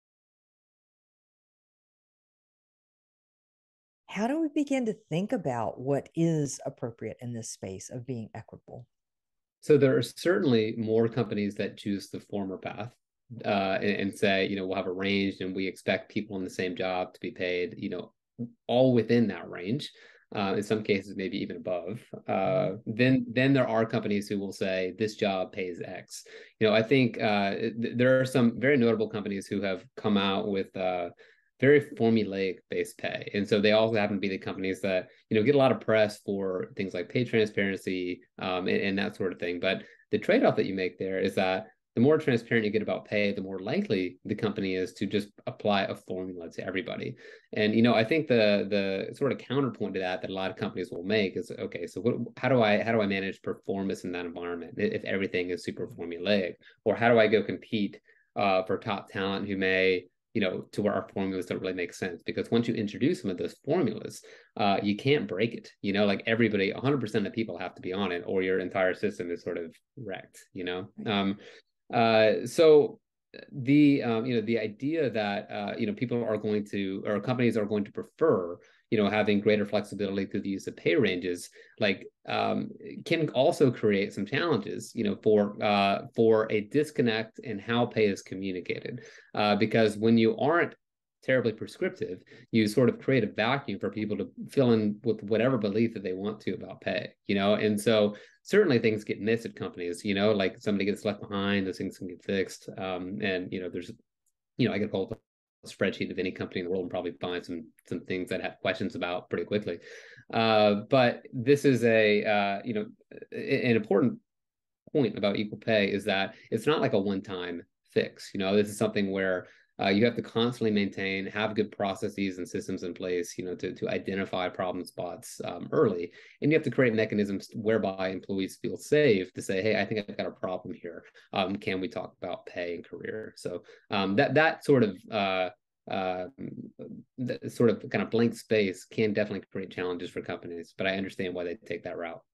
Fair Pay with Wanda Wallace and David Buckmaster
In this clip from my podcast with David Buckmaster last week, I ask David the ultimate question - what is fair pay and how do we determine it?